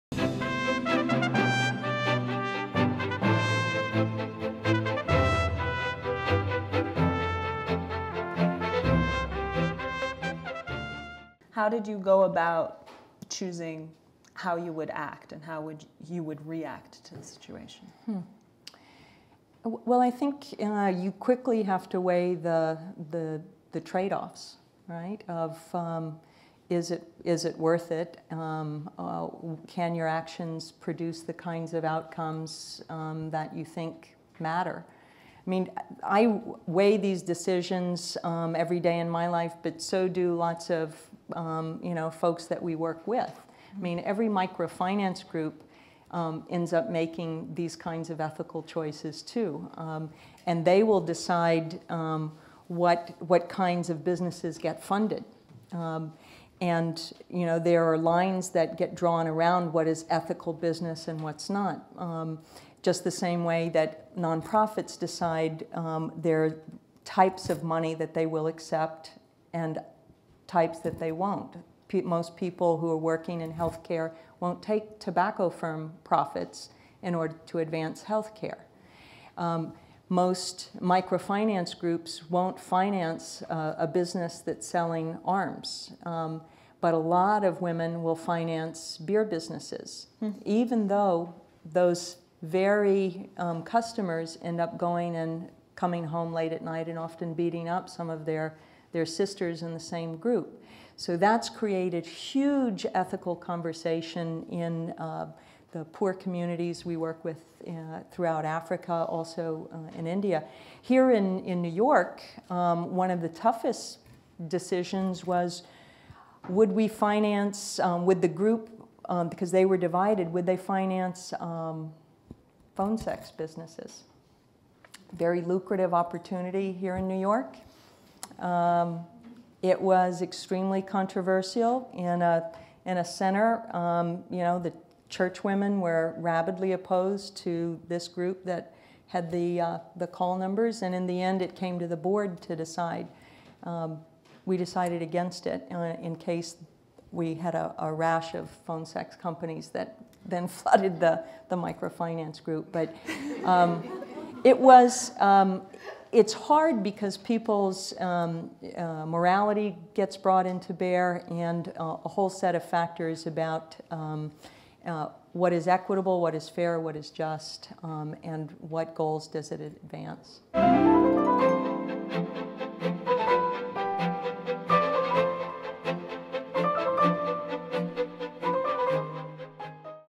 Susan Davis: Making Ethical Decisions
Microfinance pioneer Susan Davis discusses how she integrates ethics and values into her career.

This Carnegie Council event took place on June 22, 2011. For complete video, audio, and transcript, go